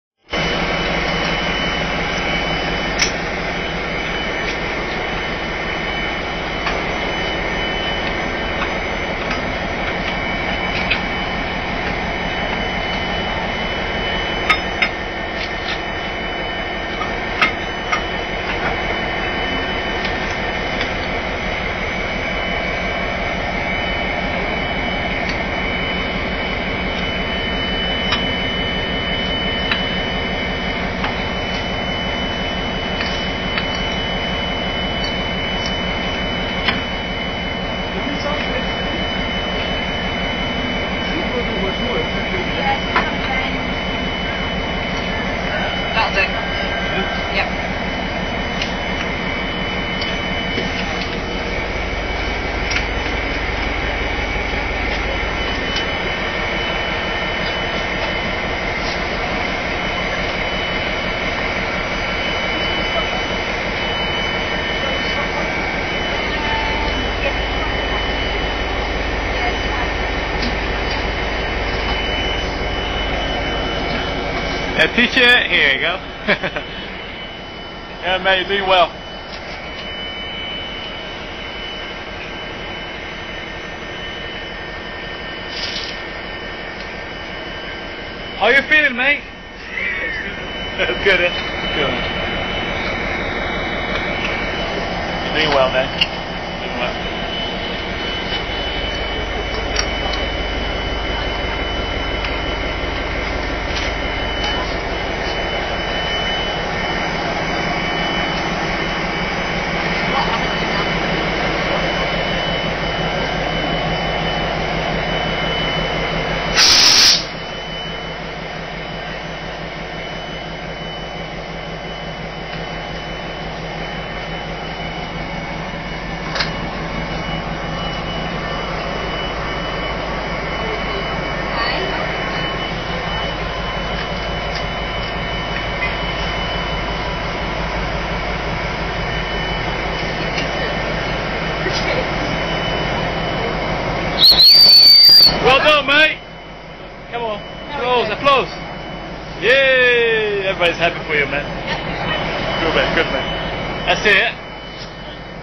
57 to 57 coupling 1
57 coupling to 57 at Longsight depot, the to locos were standing on a tight curve so the shackle was a real pain to hook on.